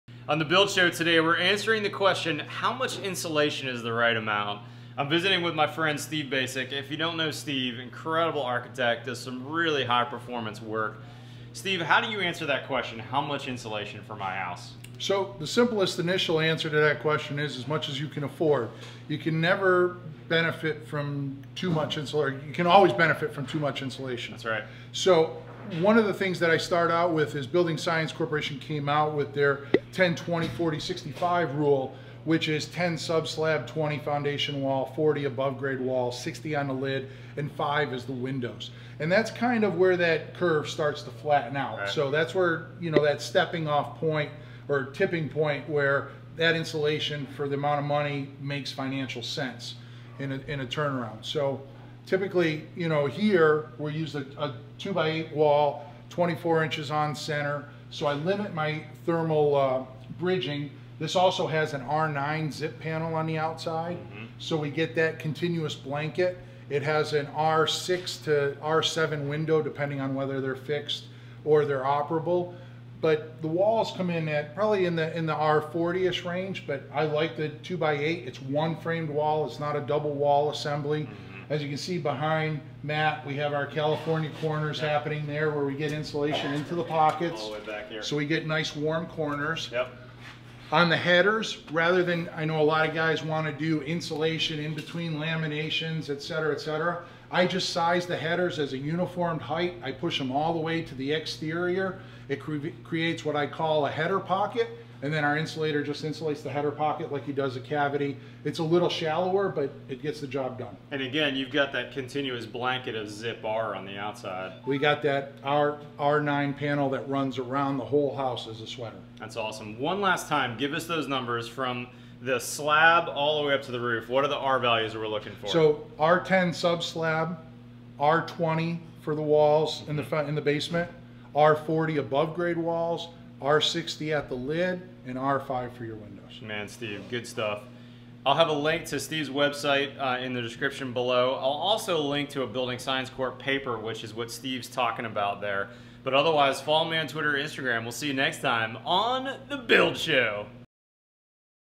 How much Insulation Do I Need? Expert advice.
Matt and Steve Baczek talk about how much insulation is the RIGHT amount in your build with a Building Science R-Value Chart!

The Build Show store on Amazon has all Matt's Favorite Tools and Books